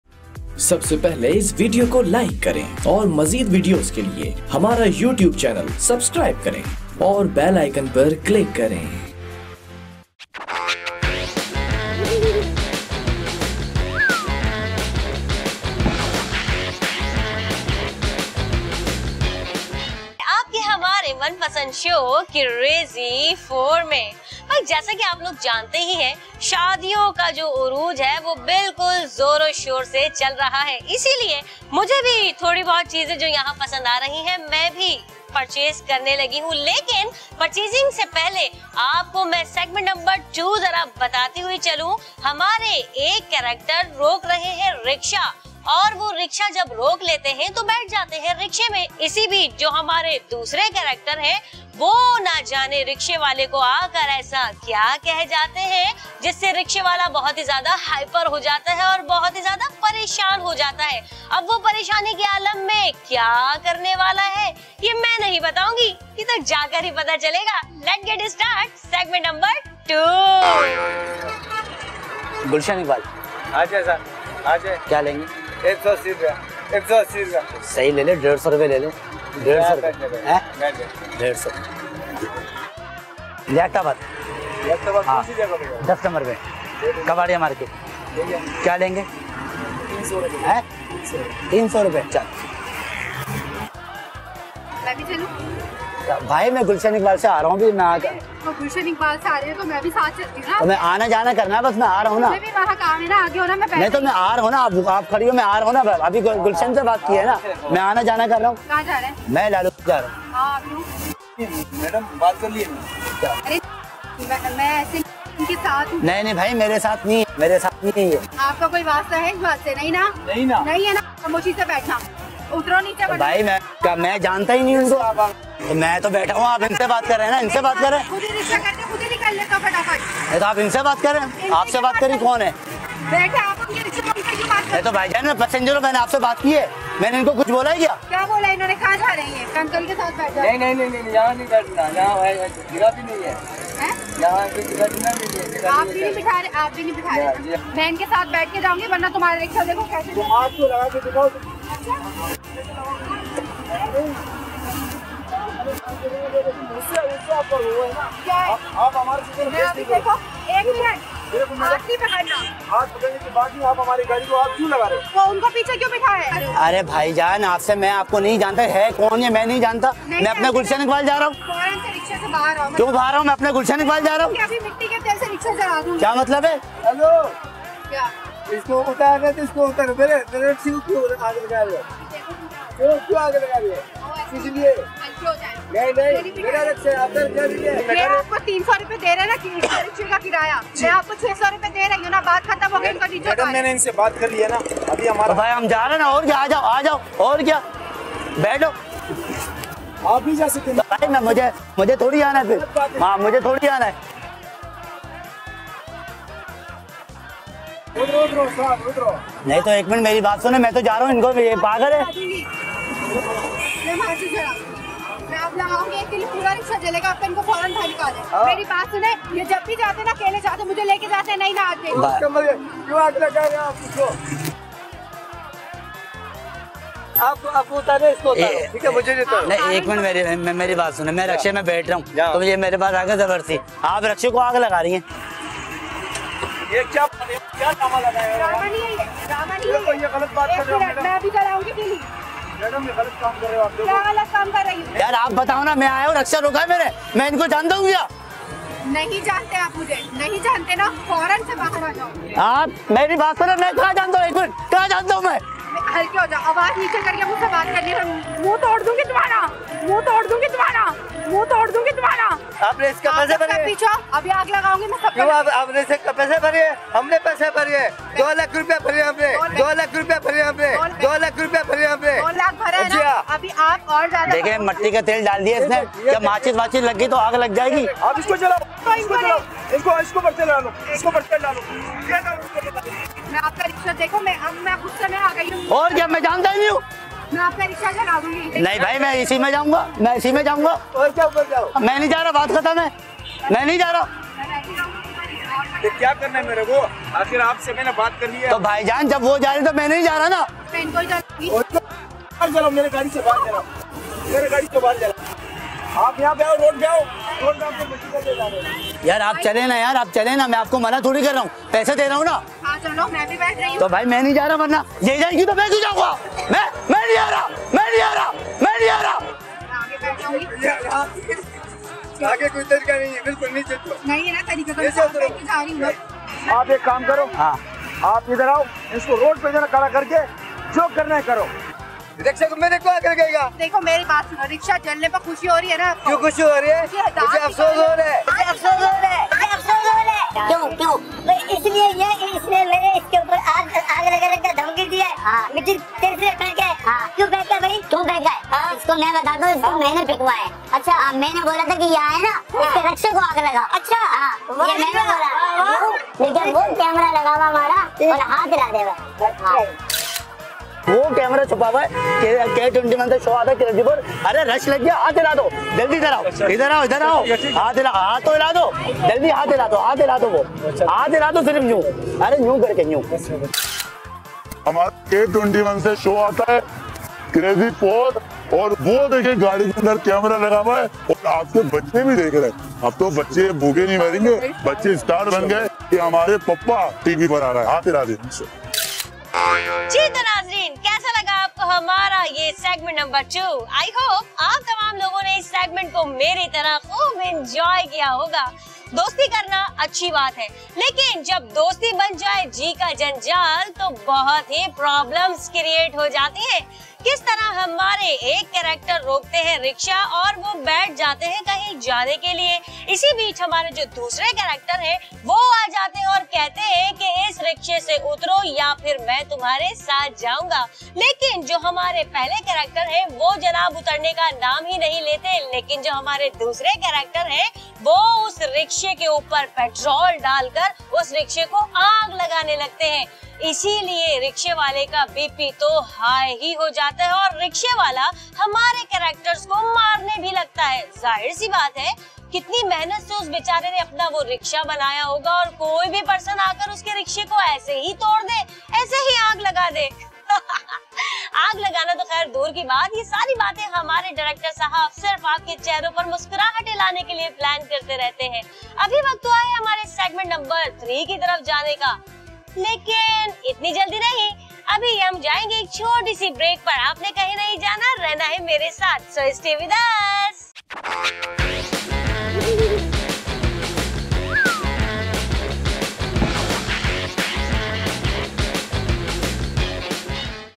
Rickshaw ko Aag Lagna Prank Krazy4 K21 News Director By Aamir Baba, 12 Jan 2020
Cameraman Ahsan
ExactiveProducer Faisal Shamsii